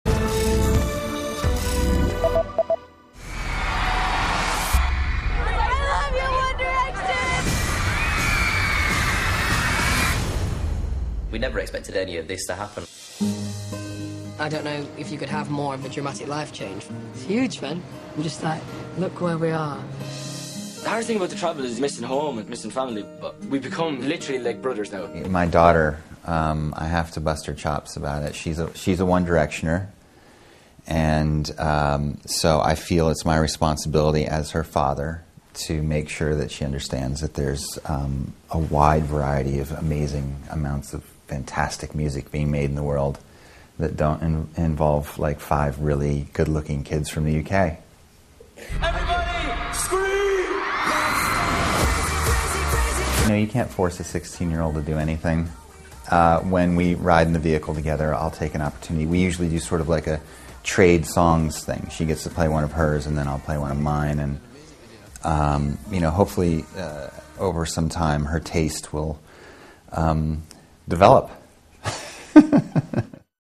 Matthew Fox Loses Daughter to One Direction
Actor Matthew Fox discusses his teenage daughter's infatuation with One Direction and reveals how he tries to steer her towards a variety of music genres. (Sept. 30)